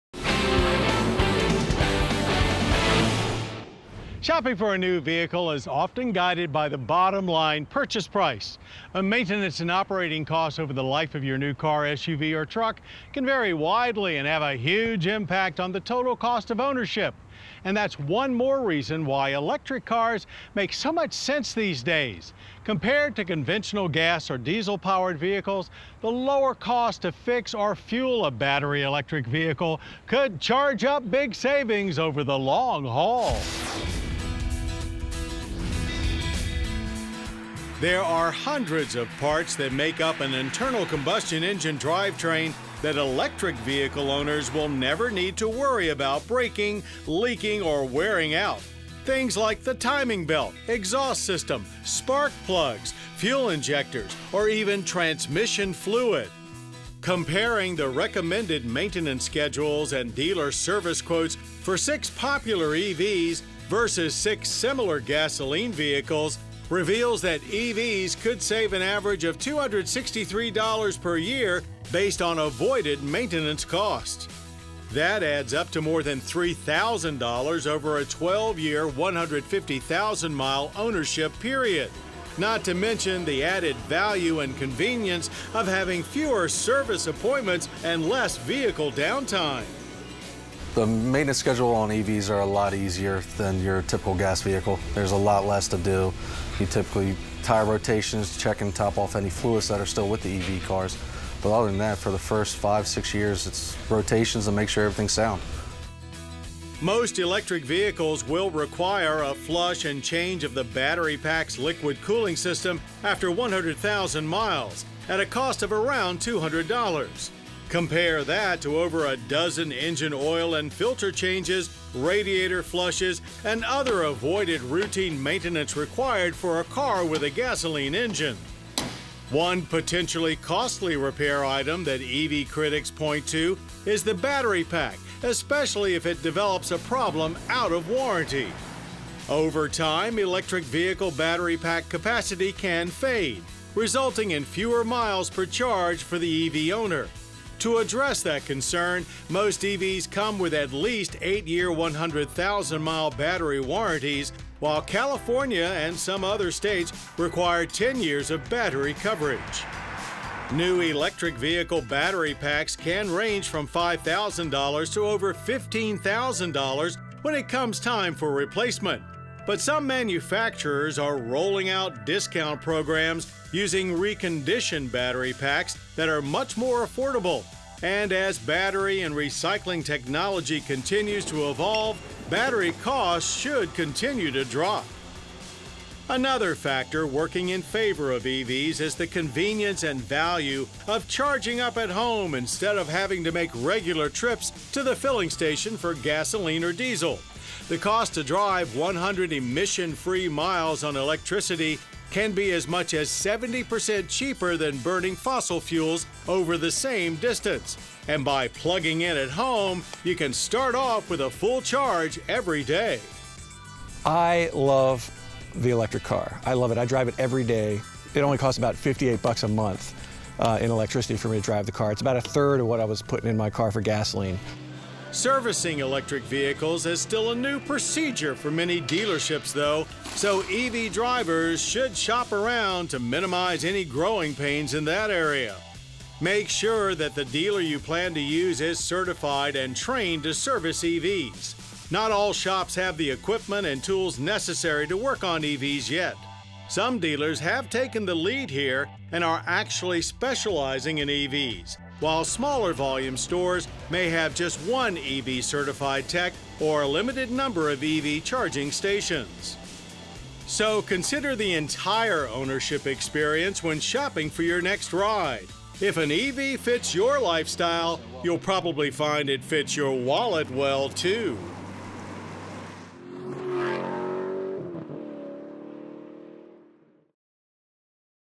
Electric vs Gas Vehicles: The Long Term Cost Comparison
Shopping for a new vehicle is often guided by the bottom line purchase price, but maintenance and operating costs over the life of your new car, SUV, or truck can vary widely and have a huge impact on the total cost of ownership. And that’s one more reason why electric cars make so much sense these days…compared to conventional gas or diesel powered vehicles the lower cost to fix or fuel a battery electric vehicle could charge up big savings over the long haul.